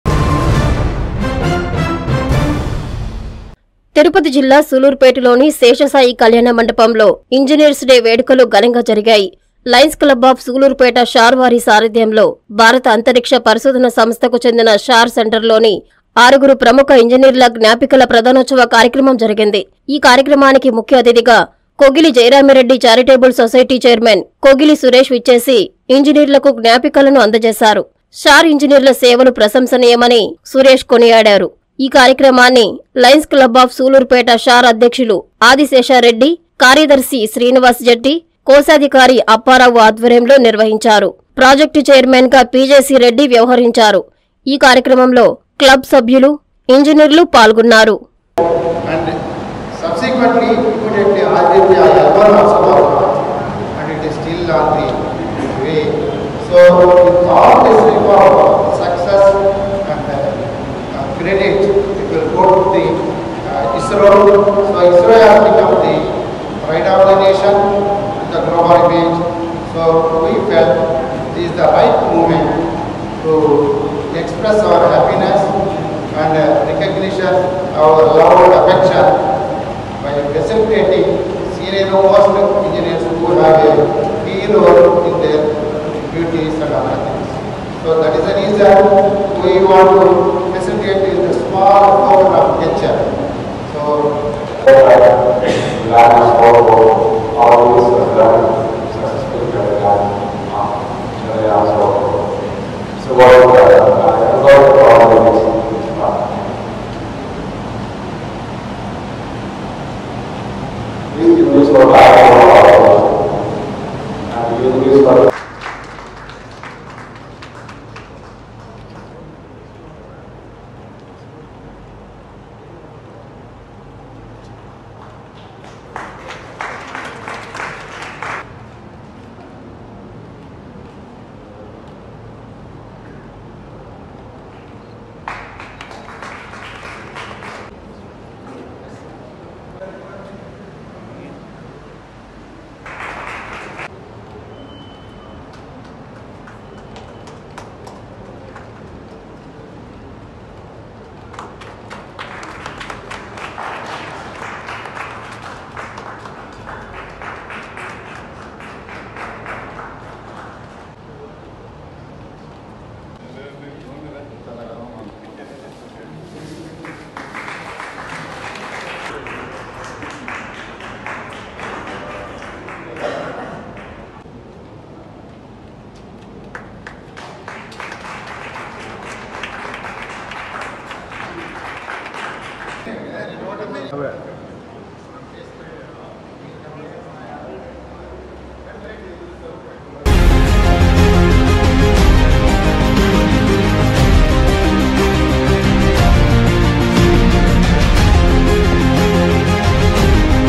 సూళ్లూరుపేట‌లో ఘ‌నంగా ఇంజ‌నీర్స్ డే@N3NEWS
ల‌య‌న్స్ క్ల‌బ్ షార్ సార‌ధ్యంలో ఇంజ‌నీర్ల‌కు స‌త్కారం